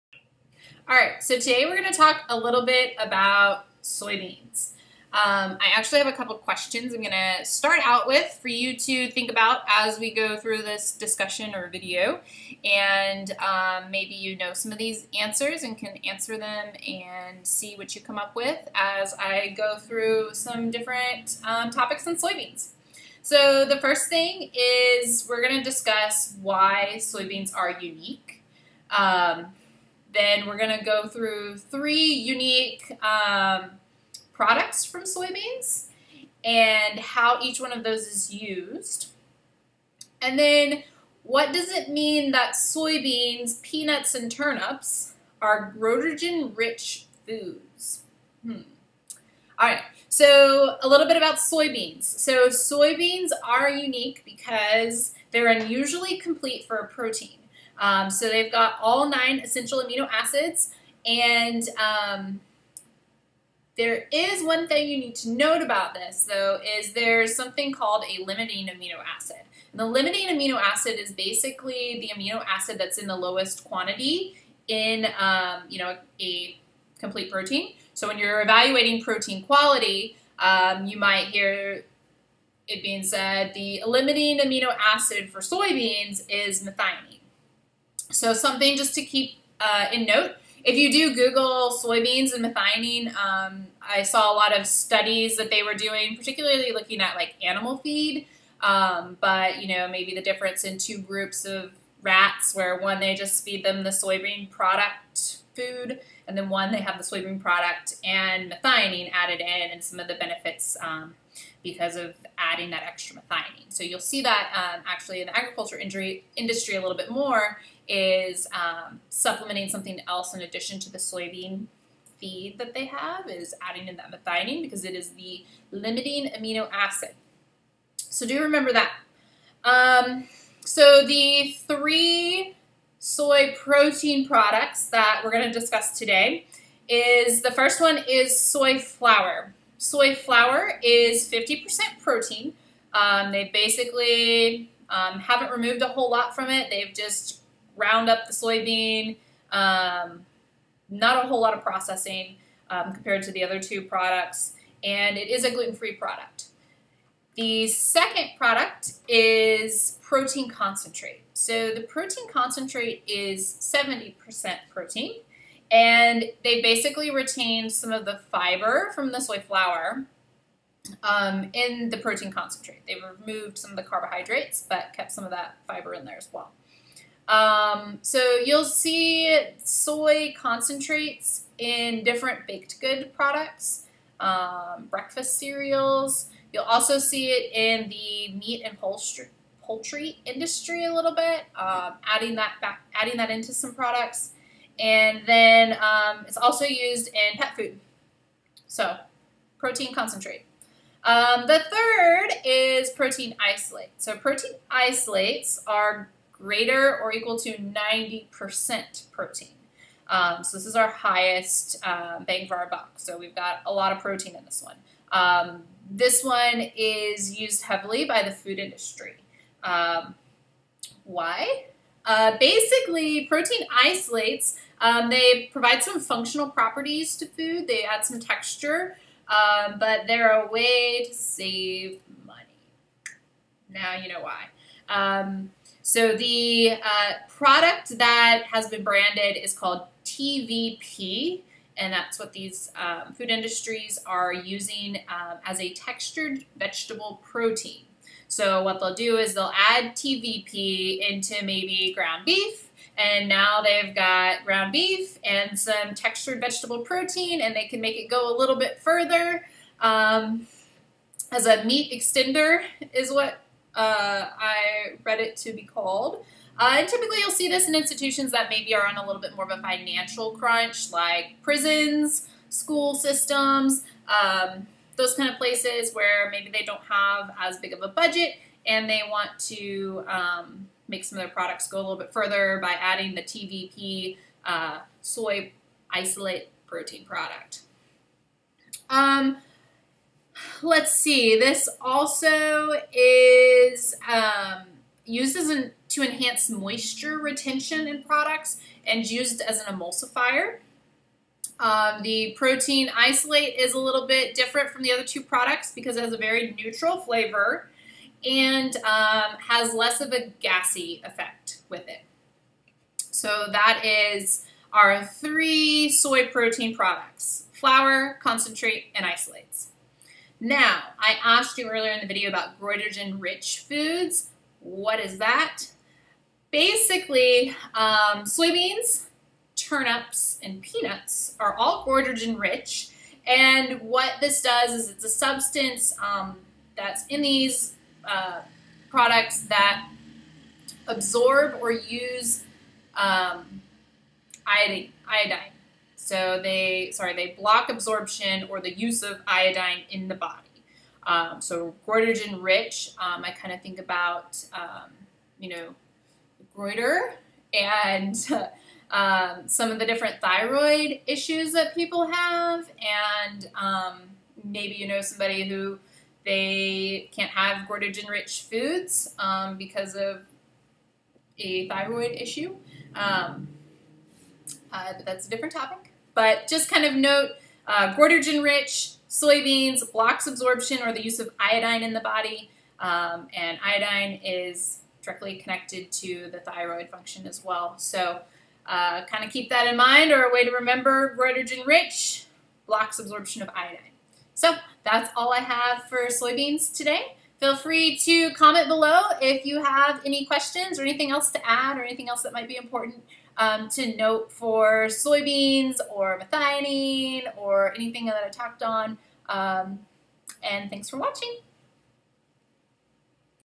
RD Exam Domain 1 Soybeans
Tips to remember!
Complete protein, limiting amino acid is methionine.
3 soy protein products include 1-soy flour (50% Pro), 2-Pro Concentrate (greater than 70% Pro), 3-Pro Isolates greater than 90% Pro.
TVP: Textured vegetable protein used by the food industry as a meat extender and for moisture retention.